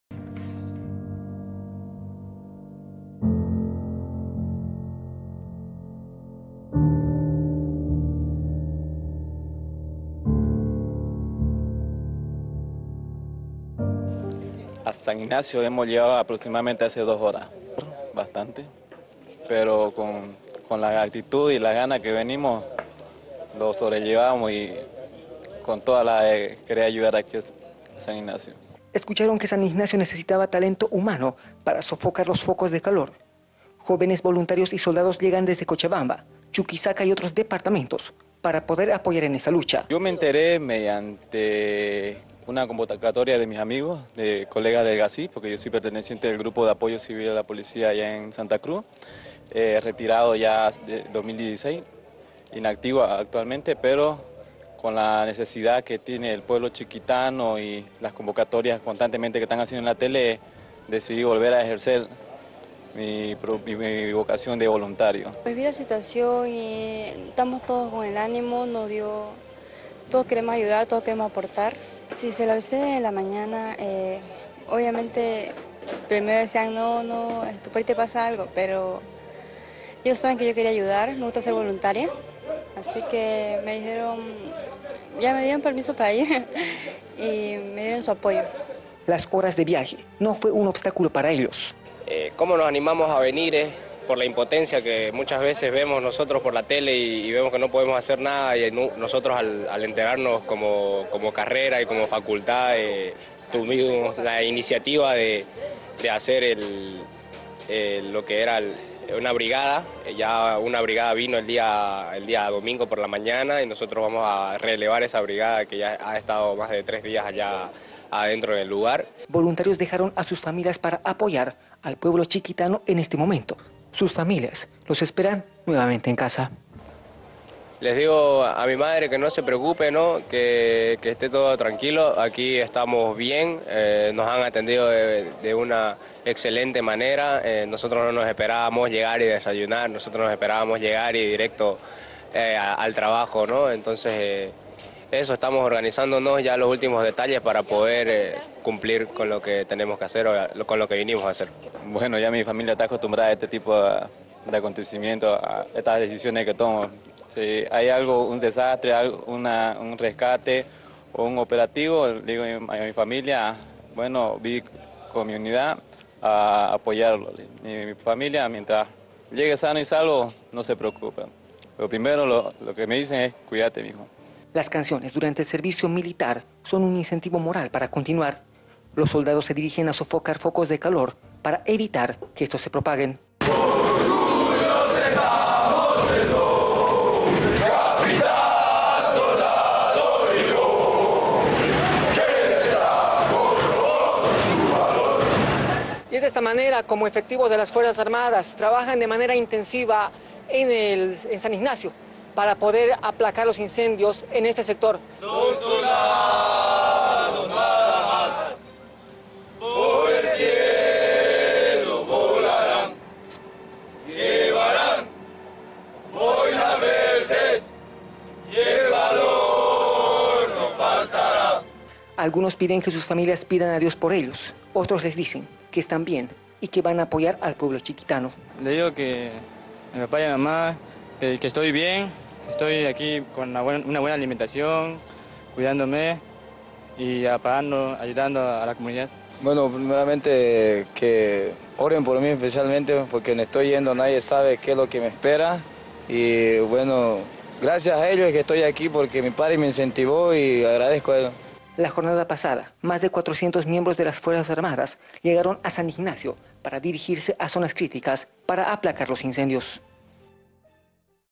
Voluntarios se suman a sofocar incendio en San Ignacio de Velasco
Jóvenes voluntarios de diferentes departamentos llegan a San Ignacio de Velasco para apoyar en la mitigación de focos de calor en la Chiquitanía.